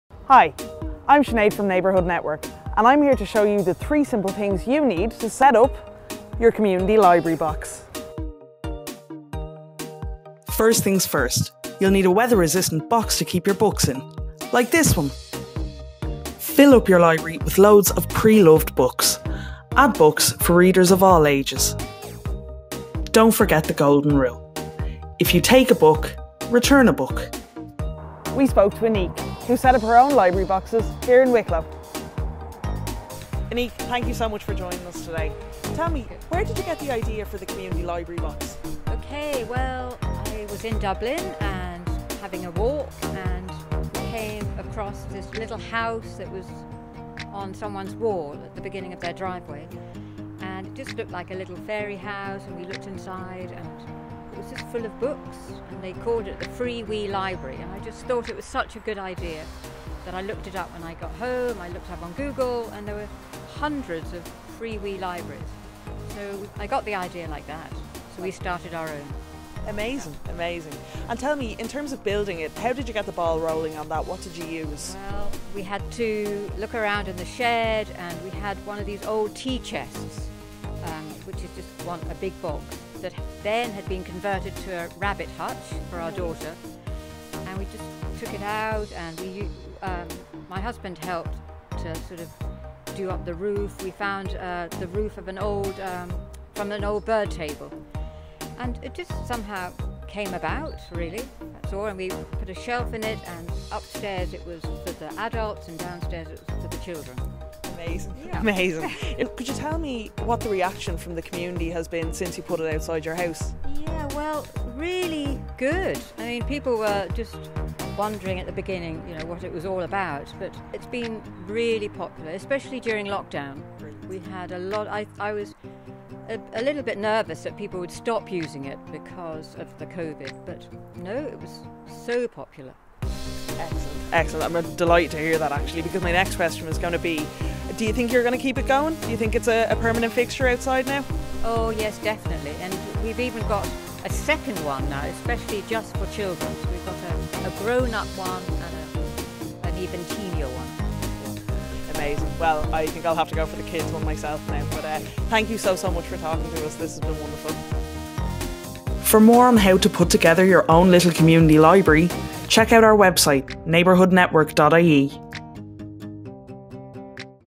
Neighbourhood Networks Guide to…Starting a Neighborhood Library Box!
A neighbourhood library box is a great way to safely connect with your neighbours and spread a little joy in your area. Sinead talks to Annick, who set up her own community library box in Co. Wicklow.